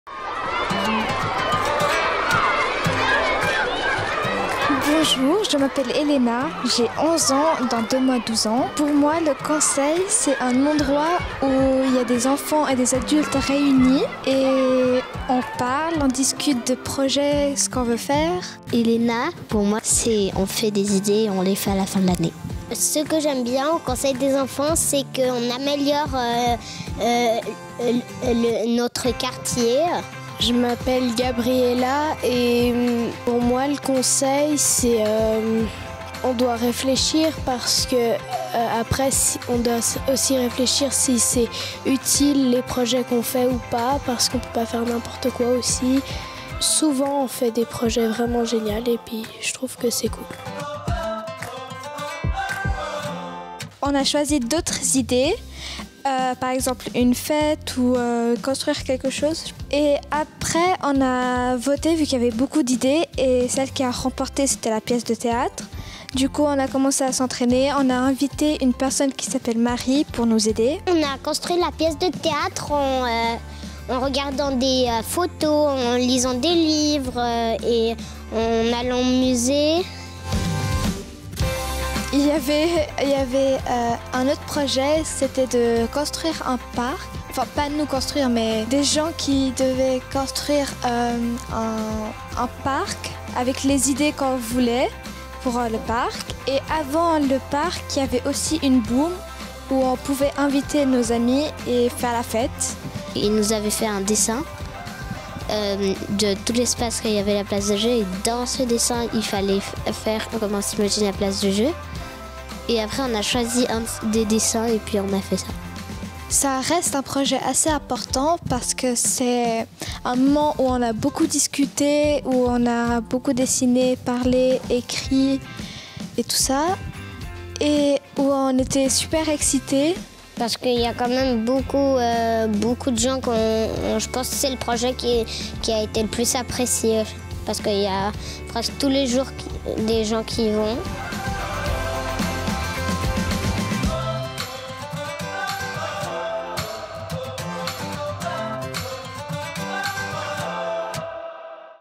Les 30 ans de la Convention des droits de l'enfant
Les Conseils des Enfants expliqués par les enfants.